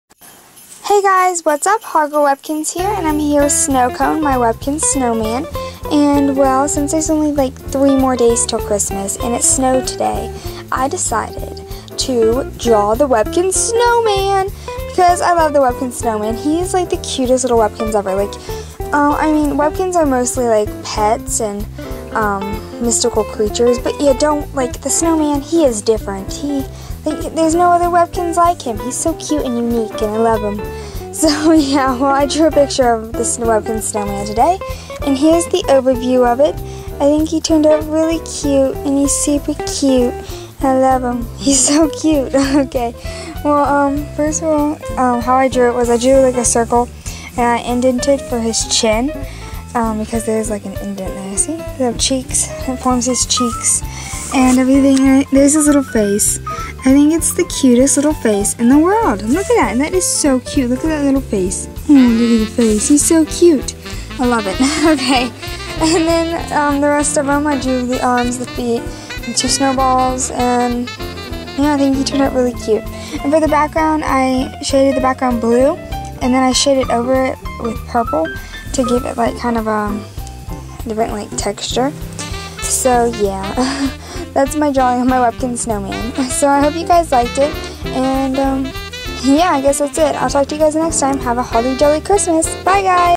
☃ Webkinz Snowman Drawing ☃
I hope you like my drawing of the webkinz snowman! Sorry I'm weird in this video, Im obsessing over how cute the webkinz snowman is. Yeah... Well Have a Holly Jolly Kinzmas!